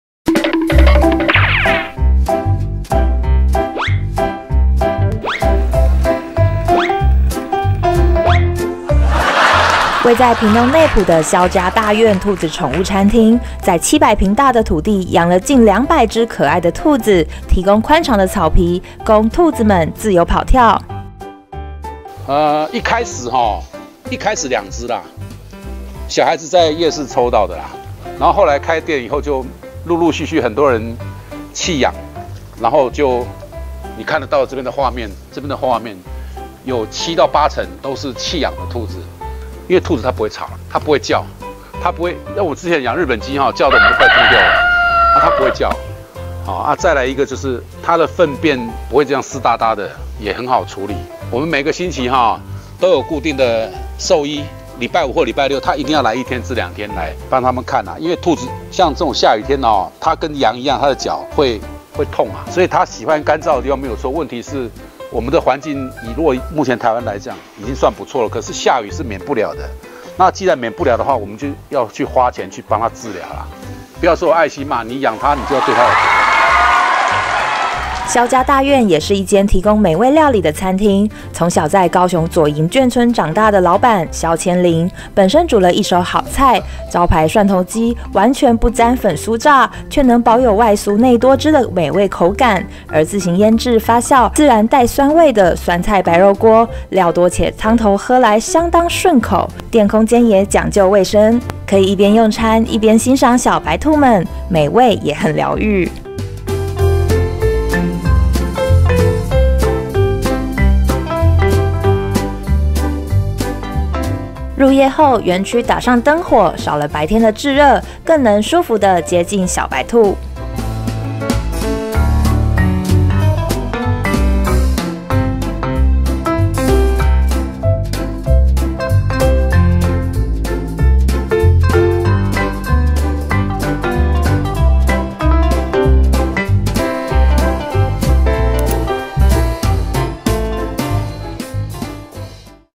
療癒系餐廳 超萌兔兔陪你一起吃飯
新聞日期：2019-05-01
屏東內埔有間餐廳，明明開在荒郊野外，但卻有不少人專門開車來吃老闆拿手的酸白菜鍋和蒜頭雞，更絕的是店裡就像一座兔子園，寬敞的草地上有近200隻萌樣破表的小白兔自由跑跳，讓客人有機會與兔子們近距離接觸！